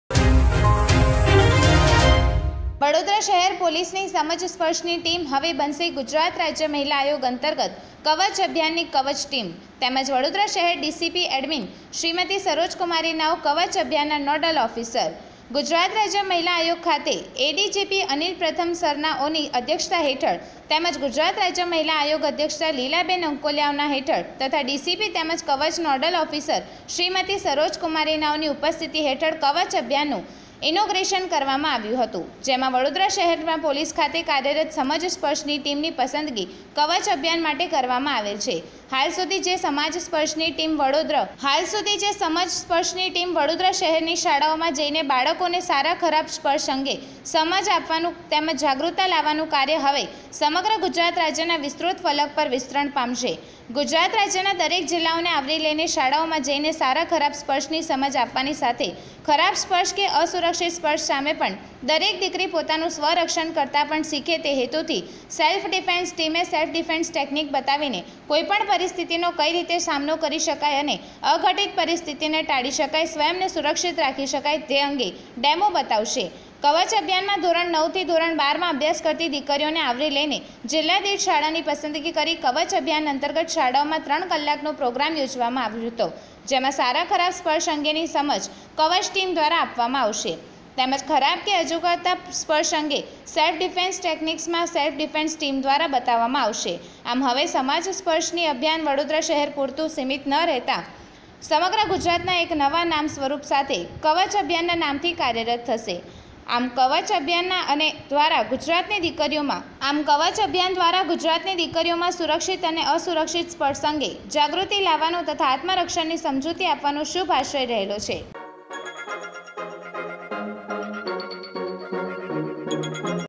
News-ગુજરાત રાજ્ય મહિલા આયોગ અંતર્ગત કવચ અભિયાન ની "કવચ ટીમ"
વડોદરા શહેર પોલીસ ની "સમજ સ્પર્શની ટીમ" હવે બનશે ગુજરાત રાજ્ય મહિલા આયોગ અંતર્ગત કવચ અભિયાન ની "કવચ ટીમ" તેમજ "વડોદરા શહેર ડીસીપી એડમીન શ્રીમતી સરોજ કુમારી નાઓ કવચ અભિયાન ના નોડલ ઓફિસર"
* આજરોજ ગુજરાત રાજ્ય મહિલા આયોગ ખાતે એ.ડી.જી.પી.અનિલ પ્રથમ સરના ઓની અધ્યક્ષતા હેઠળ તેમજ ગુજરાત રાજ્ય મહિલા આયોગ અધ્યક્ષા લીલાબેન અંકોલિયાનાઓ તથા ડી. સી. પી તેમજ કવચ નોડલ ઓફિસર શ્રીમતી સરોજકુમારીનાઓની ઉપસ્થિતિ હેઠળ કવચ અભિયાન નું આજ રોજ ઇનોગ્રેસન કરવામા આવેલ છે જેમાં વડોદરા શહેર પોલીસ ખાતે કાર્યરત સમજ સ્પર્શ ની ટીમની પસંદગી કવચ અભિયાન માટે કરવામાં આવેલ છે , હાલ સુધી જે સમજ સ્પર્શની ટીમ વડોદરા શહેરની શાળાઓમાં જઇ બાળકોને સારા ખરાબ સ્પર્શ અંગે સમાજ આપવાનું તેમજ જાગૃતતા લાવવાવનું કાર્ય હવે સમગ્ર ગુજરાત રાજ્યના વિસ્તૃત ફલક પર વિસ્તરણ પામશે, ગુજરાત રાજ્યના દરેક  જિલ્લાઓને આવરી લઇ શાળાઓમાં જઇ સારા ખરાબ સ્પર્શની સમજ આપવાની સાથે ખરાબ સ્પર્શ કે અસુરક્ષિત સ્પર્શ  સામે દરેક દીકરી પોતાનું સ્વરક્ષણ કરતા પણ શીખે તે હેતુથી સેલ્ફ ડિફેન્સ ટીમ સેલ્ફ ડિફેન્સ ટેકનીક બતાવી કોઈપણ પરિસ્થિનો કઇ રીતે સામનો કરી શકાય અને અઘટિત પરિસ્થિતિ ને ટાળી શકાય સ્વયંને સૂરક્ષિત રાખી શકાય જે અંગે ડેમો બતાવશે.કવચ અભિયાનમાં ધોરણ 9 થી ધોરણ 12 માં અભ્યાસ કરતી દીકરીઓને આવરી લઇ જીલ્લા દીઠ શાળાની પસંદગી કરી કવચ અભિયાન અંતર્ગત શાળાઓમાં 3 કલાક પ્રોગ્રામ યોજવામાં આવશે જેમાં સારા ખરાબ સ્પર્શ અંગેની સમજ કવચ ટીમ દ્વારા આપવામાં આવશે. તેમજ ખરાબ કે અજુગતા સ્પર્શ અંગે સેલ્ફ ડિફેન્સ ટેકનીકસ  સેલ્ફ ડિફેન્સ ટીમ દ્વારા બતાવવામાં આવશે આમ હવે સમજ સ્પર્શની અભિયાન વડોદરા શહેર પૂરતું સિમિતના રહેતા સમગ્ર ગુજરાતના એક નવા નામ નવા સ્વરૂપ સાથે કવચ અભિયાન ના નામ થી કાર્યરત થશે , આમ કવચ અભિયાન દ્વારા ગુજરાતની દિકરીઓમાં સુરક્ષિત અને અસુરક્ષિત સ્પર્શ અંગે જાગૃતિ લાવવાનો તથા આત્મ રક્ષણની સમજૂતી આપવાનો શુભ આશ્રય રહેલો છે.